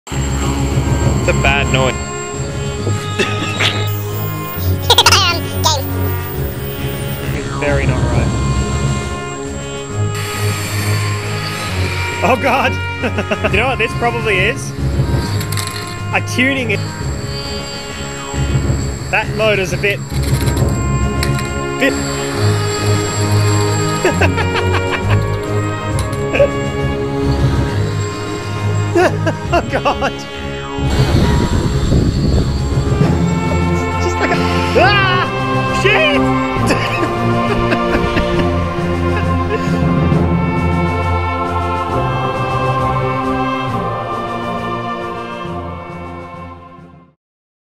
Quadcraptor?
Cam built a quadcopter and finally got all the bits he needed to fly it. I went over to render assistance. I don't think I was very good at it.